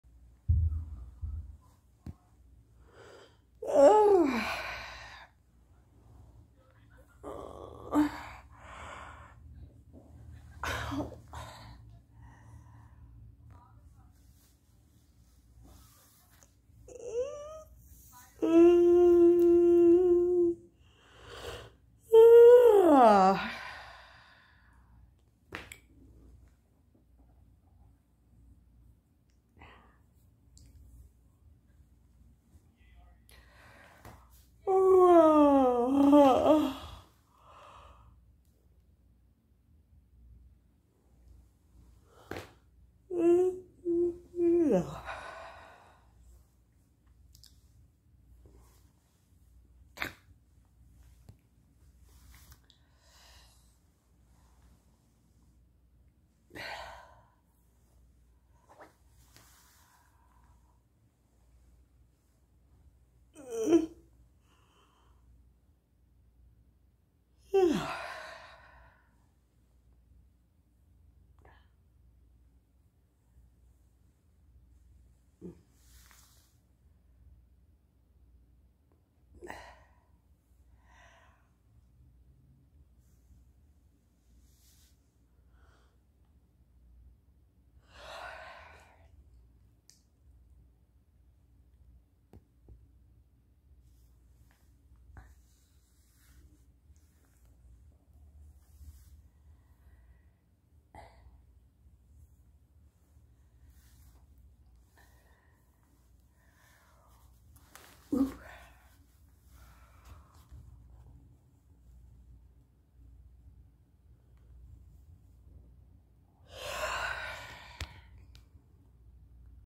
VICTOR àsmr 👅😛 WHAT DOES YOUR UVULA girl DO that Dangling thing IN BACK OF YOUR THROAT
Hi everyone

do enjoy these kind of videos I've not made in a little while